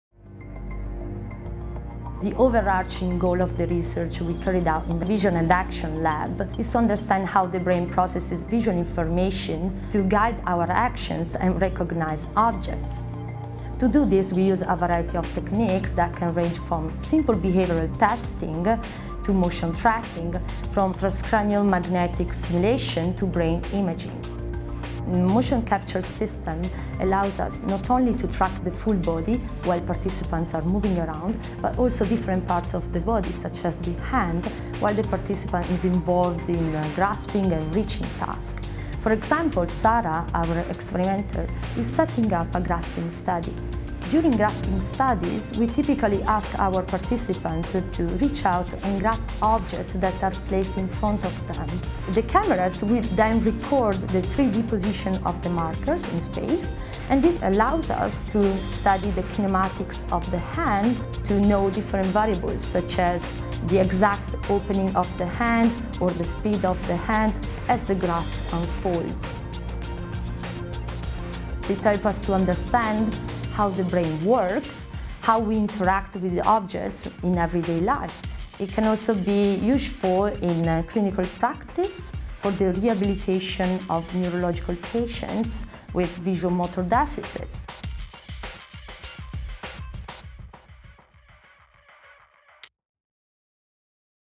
The Vision and Action Laboratory at the School of Psychology | University of East Anglia (UEA)
Dr Irene Sperandio outlines the research undertaken in the School of Psychology’s Vision & Action Laboratory and discusses the specialist equipment used.